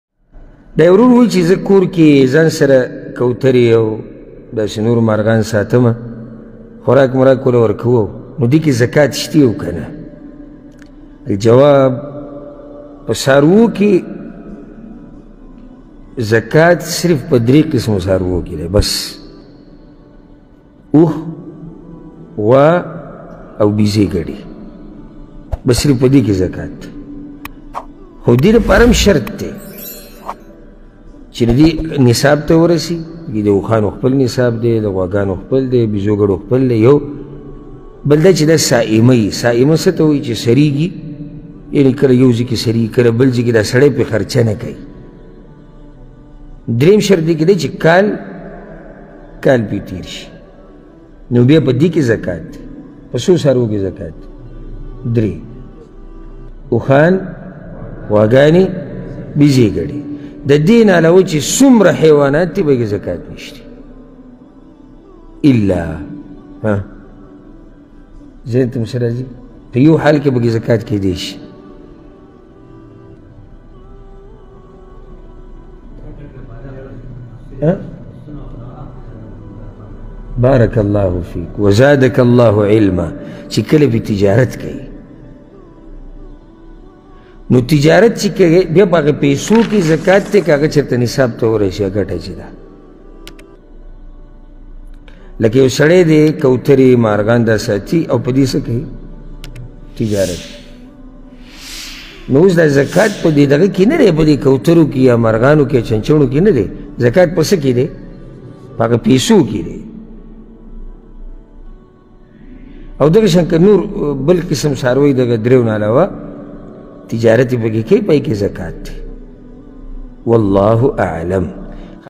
پہ کونترو او چرگانو وغیرہ کی زکوٰۃ شتہ کہ نہ یو رور سوال شیخ ابو حسان سواتی | Pa Kaotaro Ke Zakat
Pa Kaotaro Ao Chargano Ke Zakat Shta Ka Na Yaw Ror Sawal Sheikh Abu Hassan Swati.